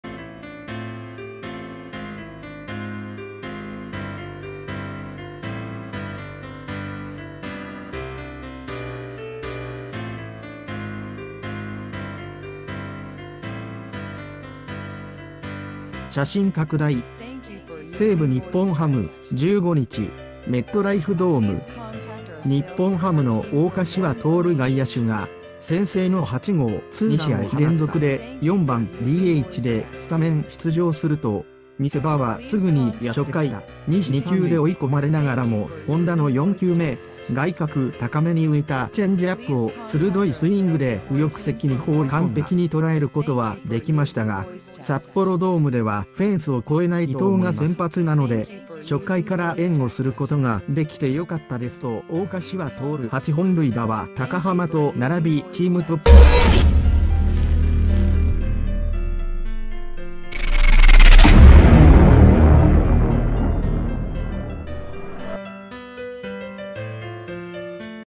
日本ハム・王柏融が先制８号２ラン でも「札幌ドームではフェンスを越えない」Top News
日本ハム・王柏融が先制８号２ラン でも「札幌ドームではフェンスを越えない」Top News 写真拡大 「西武－日本ハム」（１５日、メットライフドーム） 日本ハムの王柏融外野手が、先制の８号２ランを放った。 ２試合連続で「４番・ＤＨ」でスタメン出場すると、見せ場はすぐにやってきた。初回、２死三塁。２球で追い込まれながらも本田の４球目、外角高めに浮いたチェンジアップを鋭いスイングで右翼席に